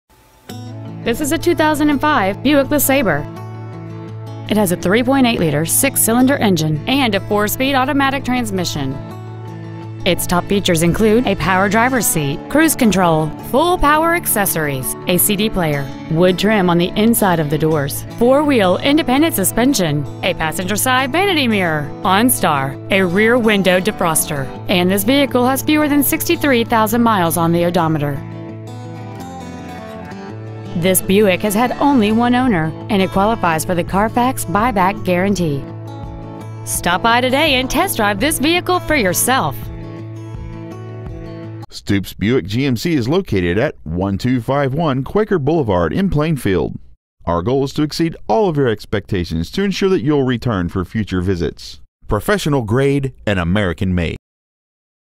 2005 Buick LeSabre Plainfield IN 46168
We are proud to present this 2005 Buick LeSabre.

 Year: 2005
 Make: Buick
 Model: LeSabre
 Engine: 3.8L V6
 Trans.: AUTO 4SPD
 Exterior: Platinum
 Miles: 62,475
 Interior: Gray